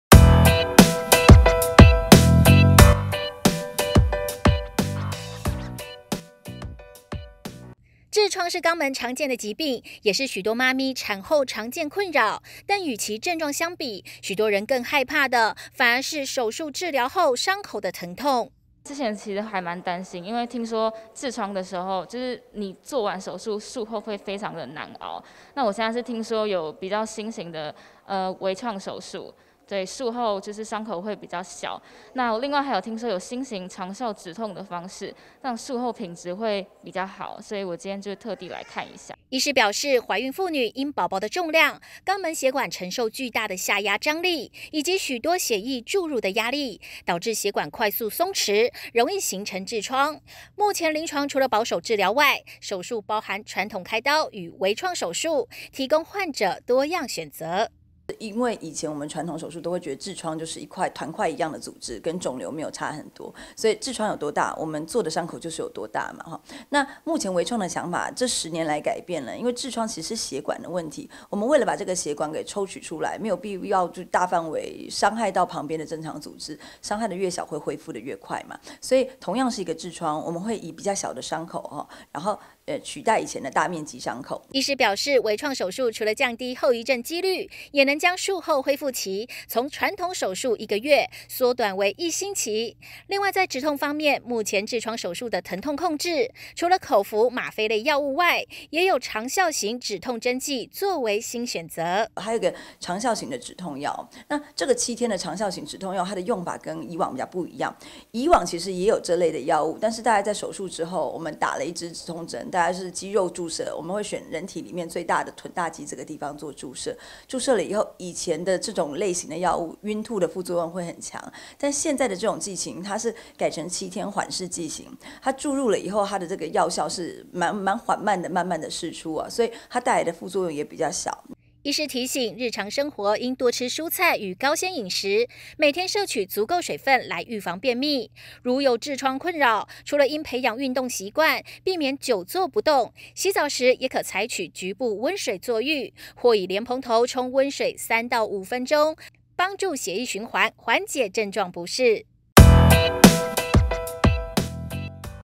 遠離產後痔瘡煩惱 微創、止痛新趨勢報到
【健康醫療網／記者林怡亭報導】痔瘡是肛門常見的疾病，也是許多媽咪產後困擾之一。禾馨民權婦幼診所醫師鍾雲霓表示，懷孕婦女因寶寶的重量，肛門血管長期承受巨大的下壓張力，以及許多血液注入的壓力，導致血管快速鬆弛，故容易形成痔瘡。目前臨床除了保守治療外，也可進行外科手術切除。然而與痔瘡症狀相比，許多患者往往更害怕的是術後傷口可能帶來的疼痛。

痔瘡, 鍾雲霓